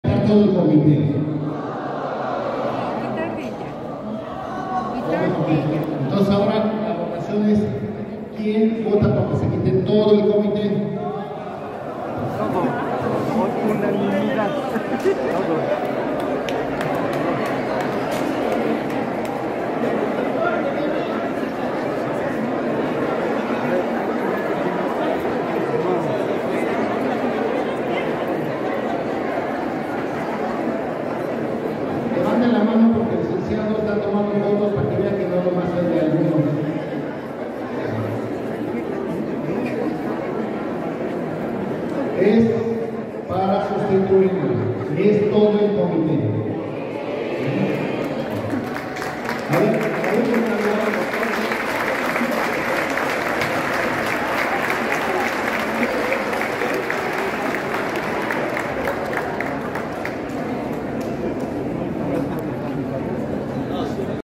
Trabajadores sindicalizados del ISSSTEP destituyeron a su dirigente Maribel Lázaro Juárez
En presencia de la Segob y notario público, este sábado en sesión extraordinaria trabajadores sindicalizados del ISSSTEP destituyeron a su dirigente Maribel Lázaro Juárez